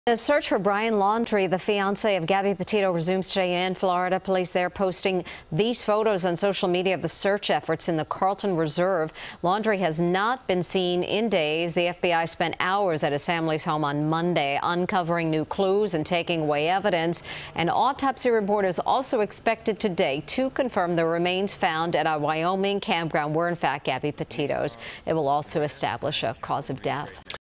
Search Resumes For Brian Laundrie In Florida
Authorities continue to search for the fiancé of Gabby Petito, as the autopsy results are expected on a body believed to be hers.